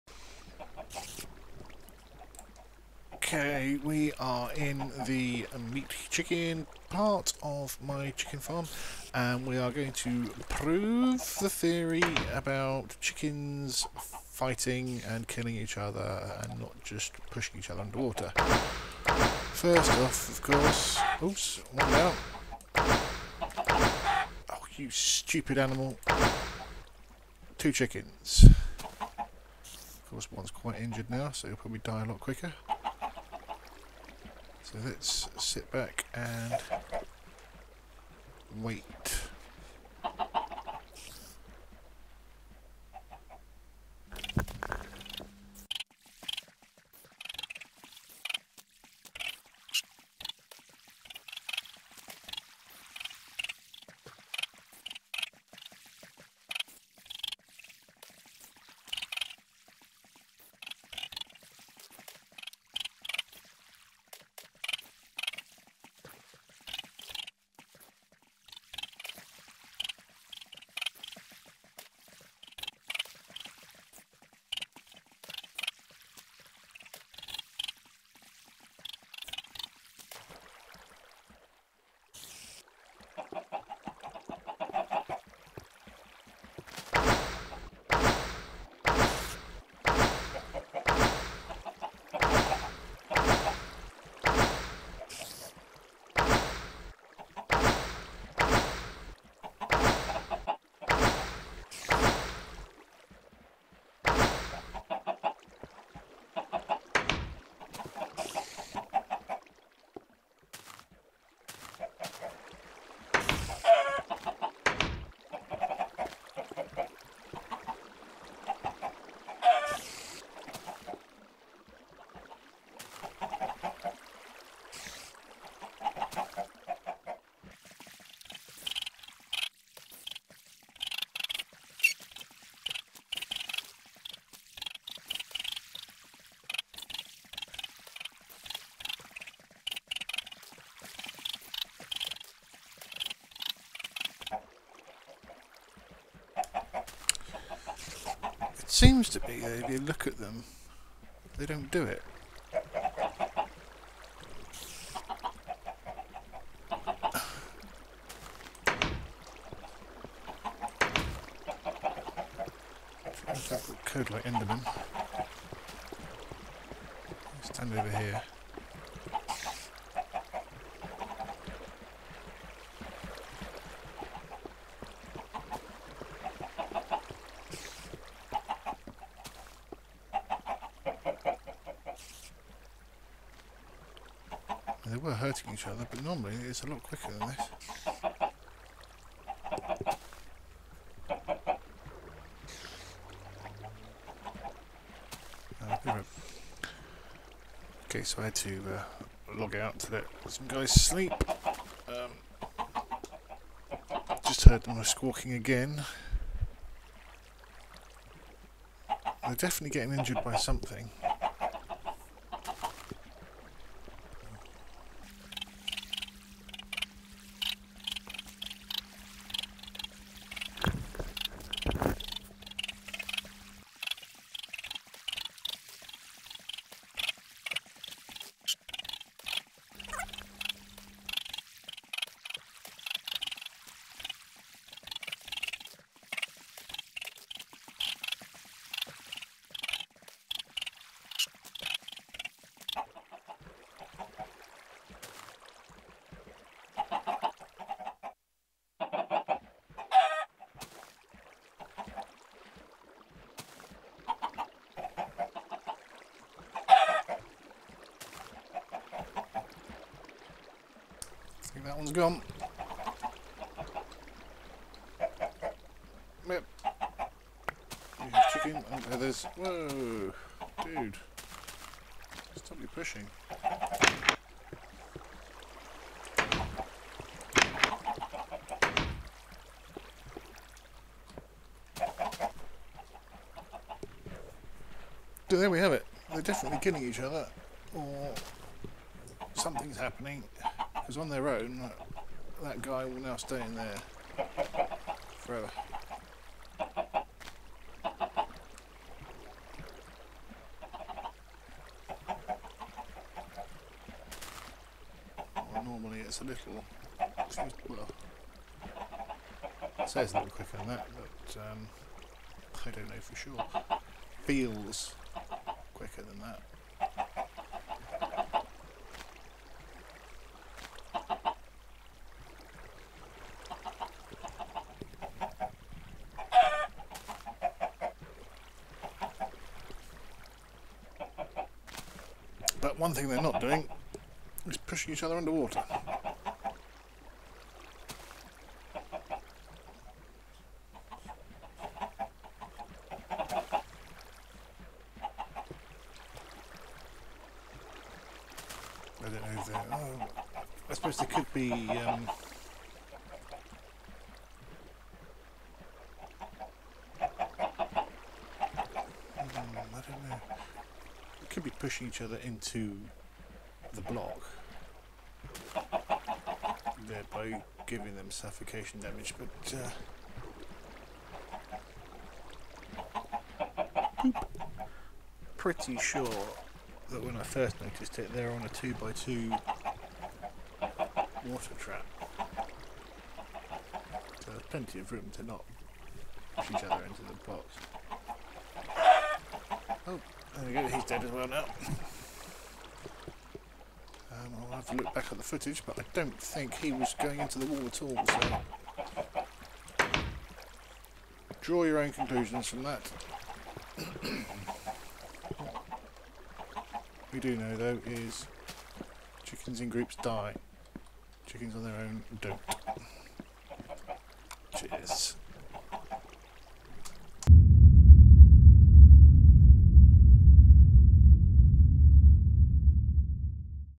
Chicken theory
Testing to see if multiple chickens in close confinement kill each other.